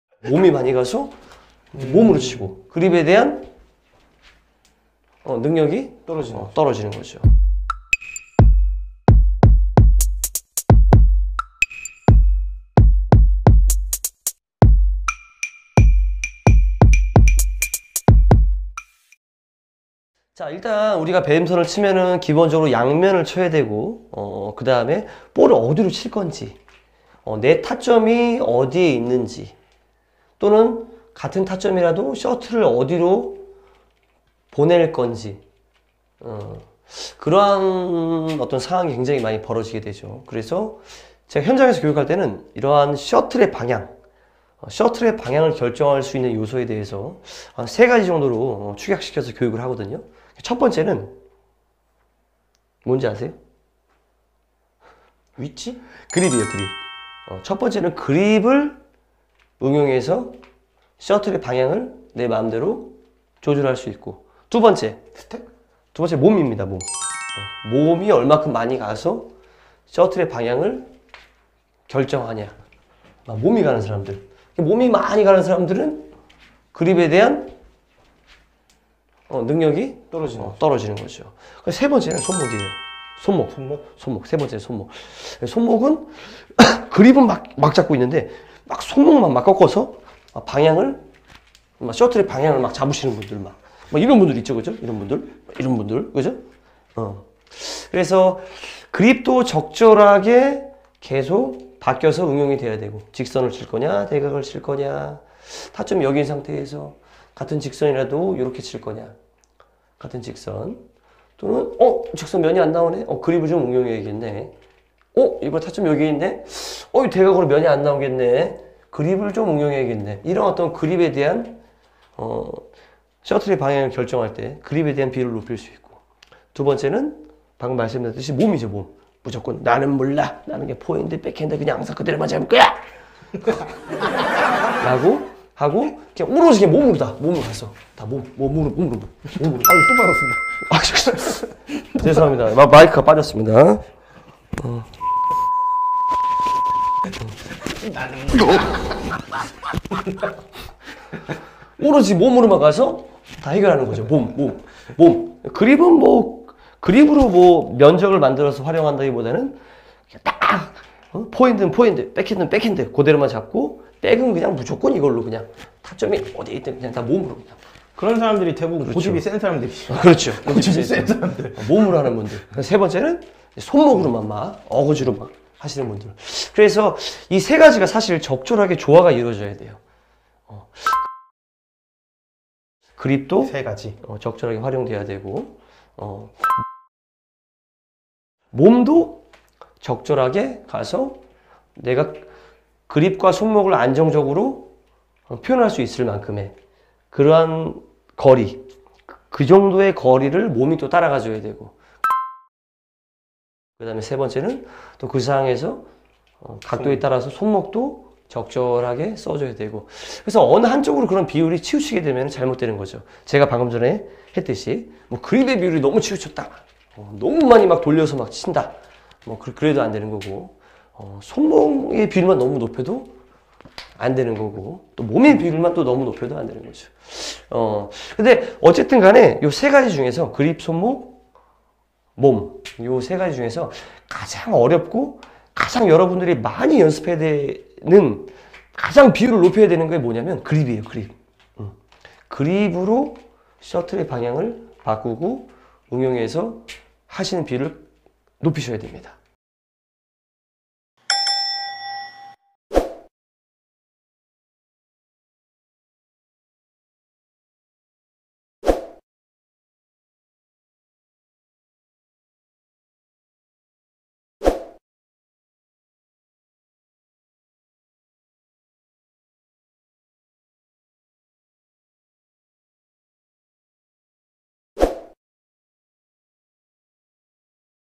배드민턴 셔틀의 방향을 결정하는 요소 3가지 [집콕민턴]
배드민턴은 고정적인 운동이 아닌
유동적인 운동입니다. 기초단계를 넘어가면
다양한 상황에 따라 자신만의 스텝리듬, 기술적인 테크닉,
그리고 다양한 스윙의 형태가 만들어져야 합니다.
더콕은 배드민턴의 응용학습을 지향합니다.